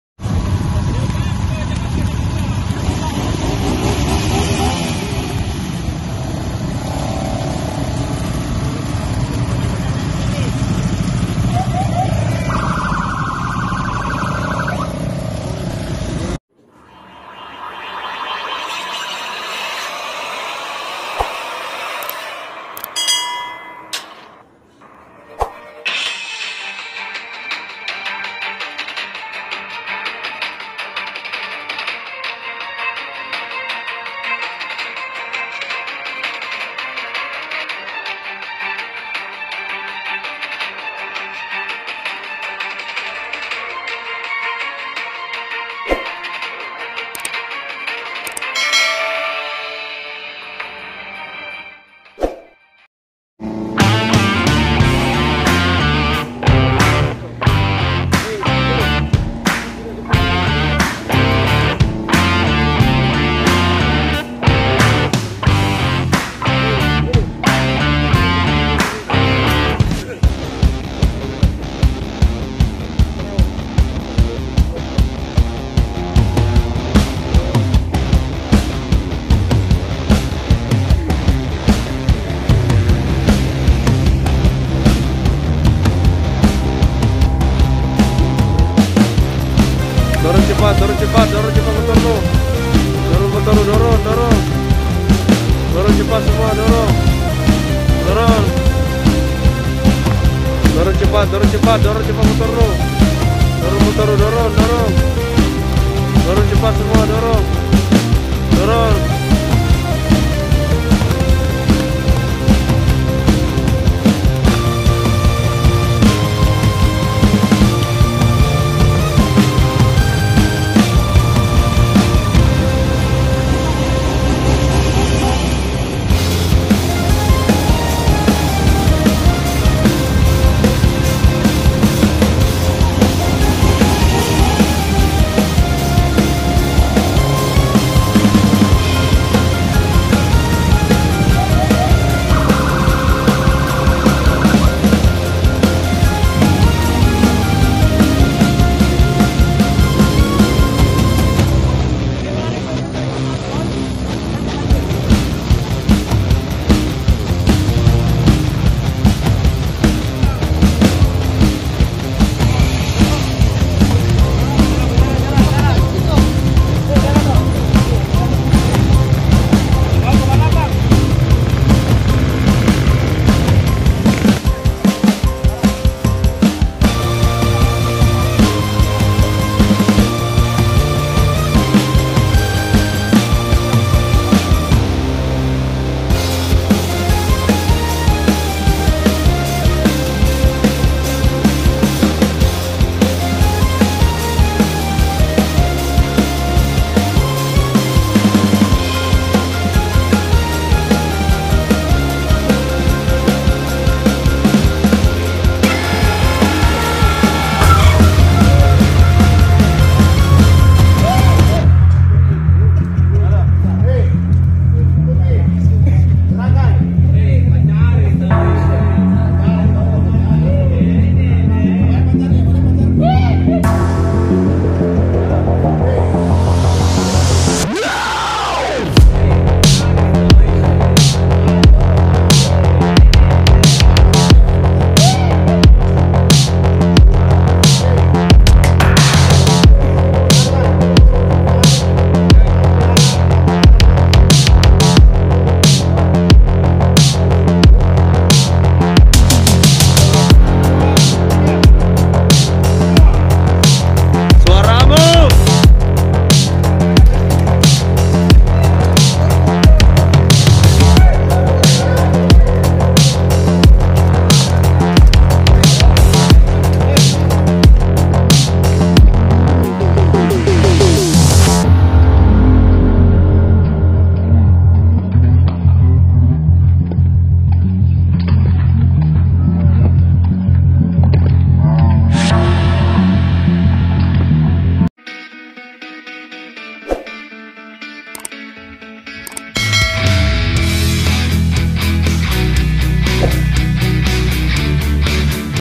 EFEK JERA PARA REMAJA YANG TERLIBAT BALAP LIAR
Saksikan Video sebelumnya saat Tim Penikam Terlibat Aksi Kejar Kejaran dengan Geng Motor